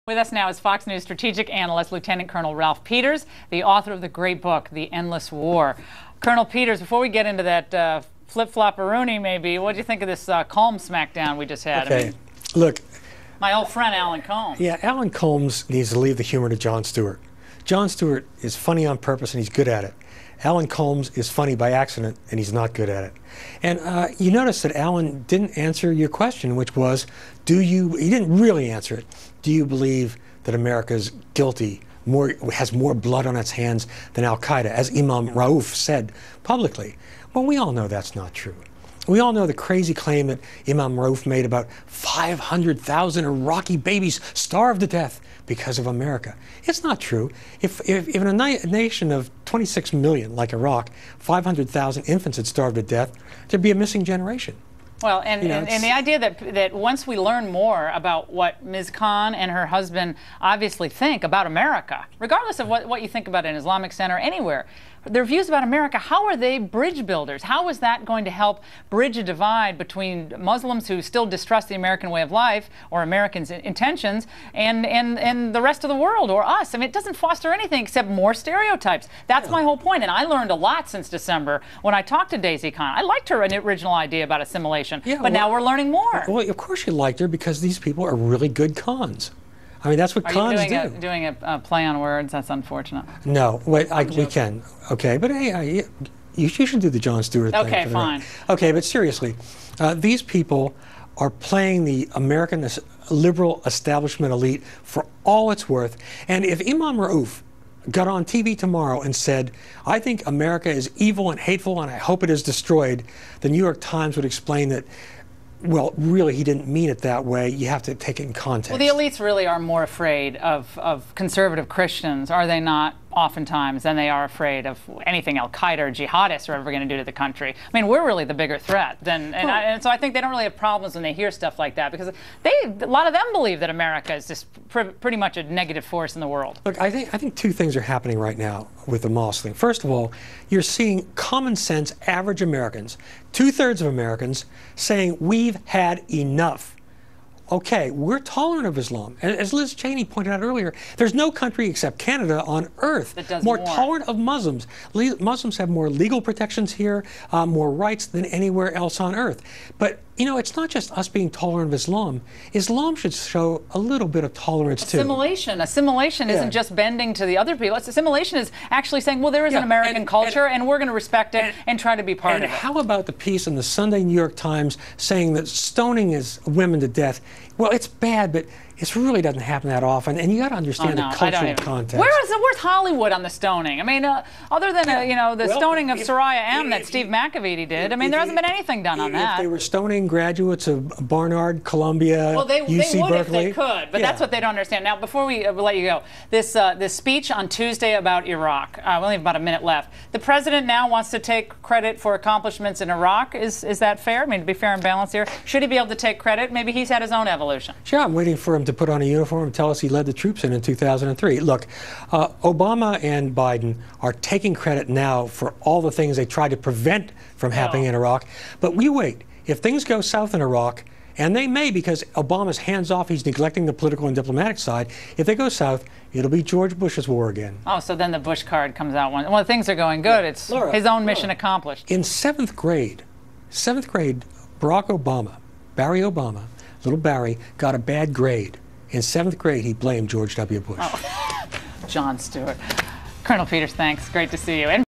Lt. Col. Ralph Peters: Allen Colmes not funny
Oriellys Allen Colmes did not really answer question "do you believe America has more blood on its hands then AlQaeda?"